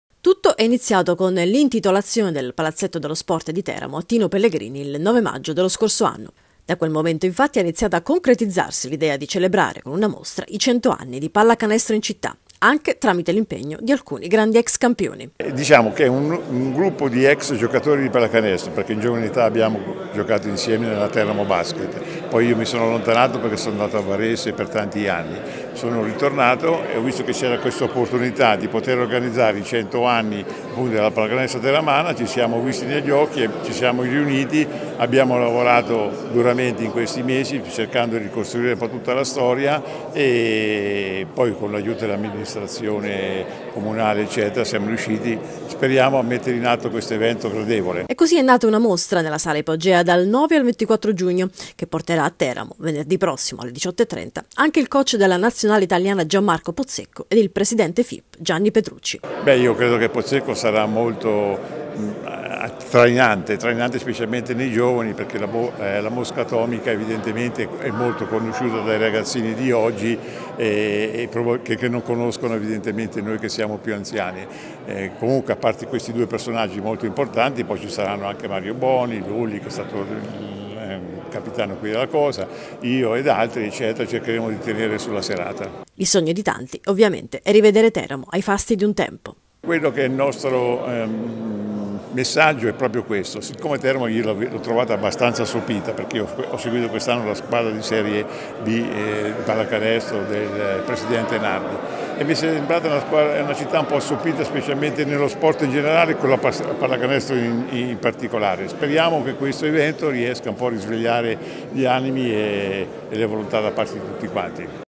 A Teramo si celebrano i cento anni di pallacanestro
Dal 9 al 24 giugno mostra alla sala Ipogea a Teramo sui cento anni di pallacanestro teramana. Venerdì inaugurazione con Gianmarco Pozzecco, coach della nazionale italiana, ed il presidente della Federazione Pallacanestro, Gianni Petrucci